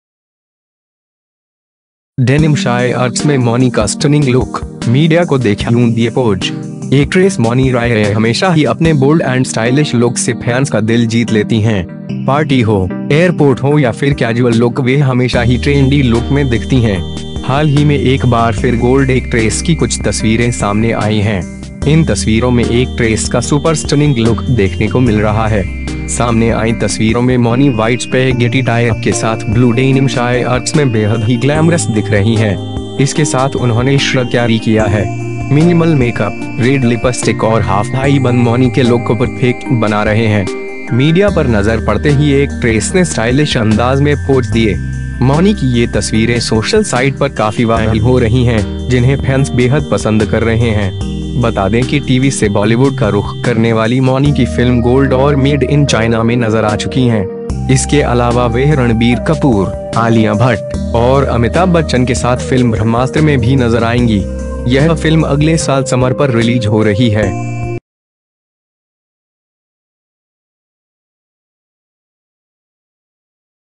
डेनिम शाॅर्ट्स में मौनी का स्टनिंग लुक, मीडिया को देख यूं दिए पोज
एक्ट्रेस मौनी राॅय हमेशा ही अपने बोल्ड एंड स्टाइलिश लुक्स से फैंस का दिल जीत लेती हैं। पार्टी हो, एयरपोर्ट हो या फिर कैजुअल लुक वह हमेशा ही ट्रेंडी लुक में दिखती हैं। हाल ही में एक बार फिर गोल्ड एक्ट्रेस की कुछ तस्वीरें सामने आईं हैं। इन तस्वीरों में एक्ट्रेस का सुपर स्टनिंग लुक देखने को मिल रहा है। सामने आईं तस्वीरों में मौनी व्हाइट स्पैगिटी टाॅप के साथ ब्लू डेनिम शाॅर्ट्स में बेहद ही ग्लैमरस दिख रही हैं। इसके साथ उन्होंने श्रग कैरी किया है। मिनिमल मेकअप,रेड लिपस्टिक और हाफ हाई बन मौनी के लुक को परफेक्ट बना रहे हैं। मीडिया पर नजर पड़ते ही एक्ट्रेस ने स्टाइलिश अंदाज में पोज दिए। मौनी की ये तस्वीरें सोशल साइट पर काफी वायरल हो रही हैं, जिन्हें फैंस बेहद पसंद कर रहे हैं।

बता दें कि टीवी से बॉलीवुड का रुख करने वाली मौनी की फिल्म 'गोल्ड' और 'मेड इन चाइना' में नजर आ चुकी हैं। इसके अलावा वह रणबीर कपूर, आलिया भट्ट, और अमिताभ बच्चन के साथ फिल्म 'ब्रह्मास्त्र' में भी नजर आएंगी। यह फिल्म अगले साल समर पर रिलीज हो रही है।